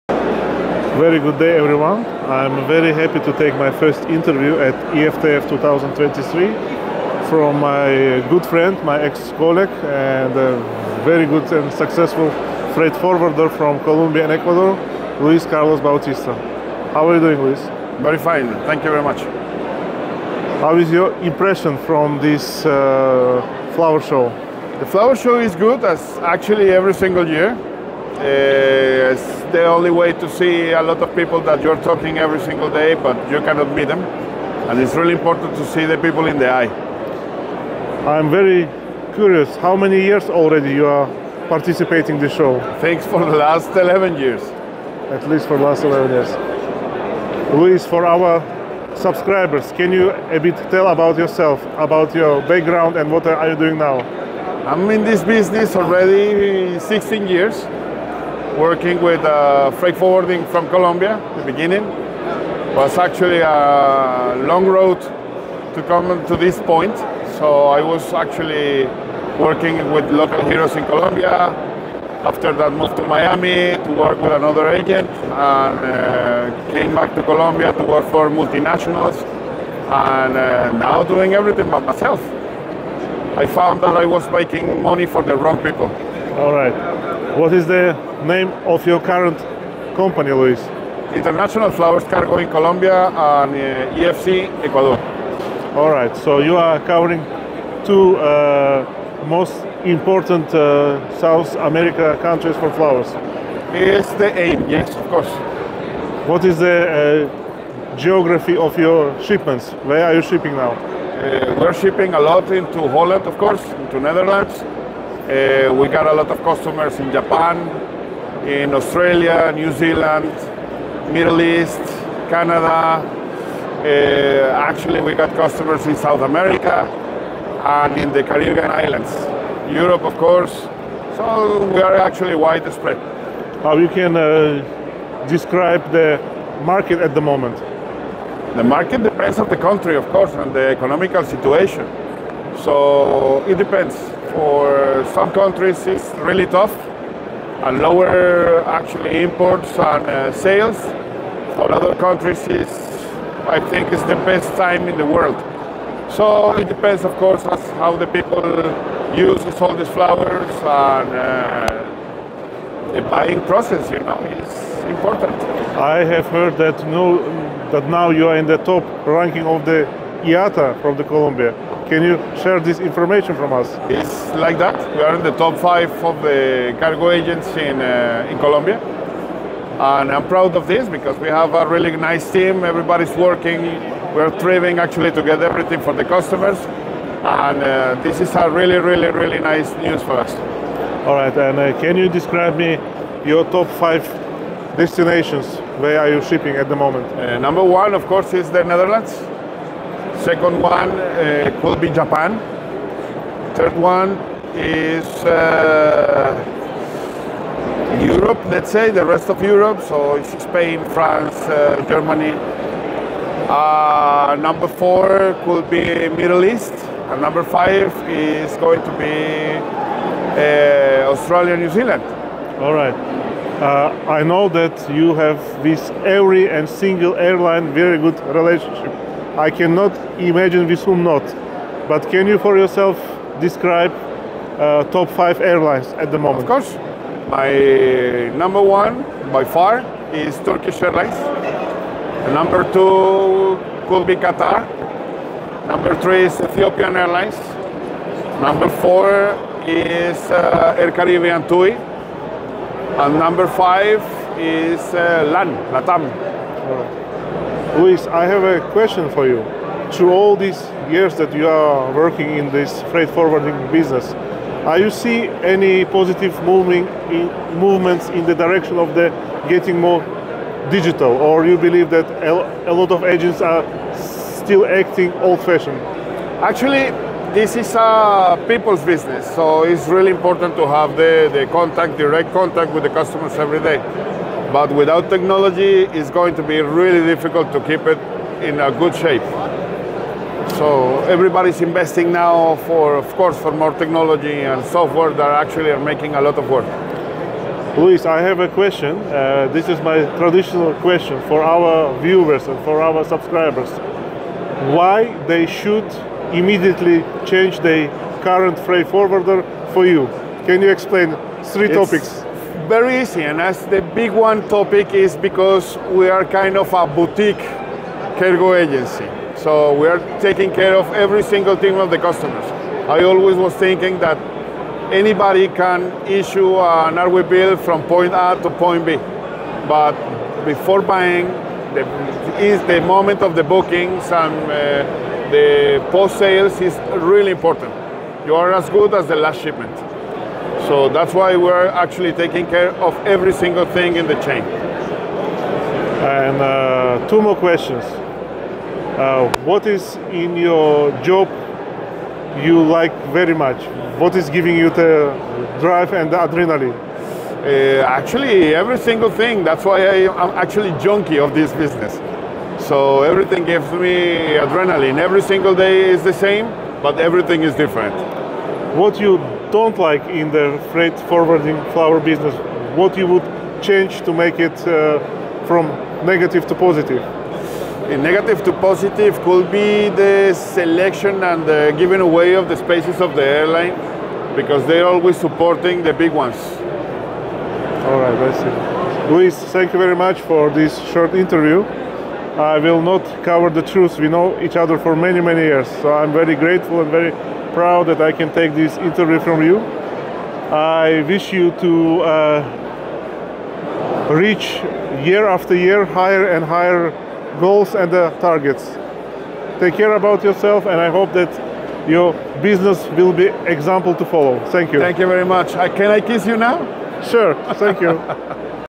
IFTF 2023. Interview with Luis Carlos Bautista, IFC Colombia/Ecuador
Good day, dear friends! Today we are delighted to bring you an interview with Mr. Luis Carlos Bautista, a successful freight forwarder from Colombia and Ecuador, at the IFTF 2023 - International Floriculture Trade Fair. In this interview, we will hear his impressions of the fair, his 16 years of experience in the business, and how his company, International Flowers Cargo, is using innovations to improve the quality of transportation and expand its delivery geography, including to the Netherlands, Japan, Australia, New Zealand, and the Middle East.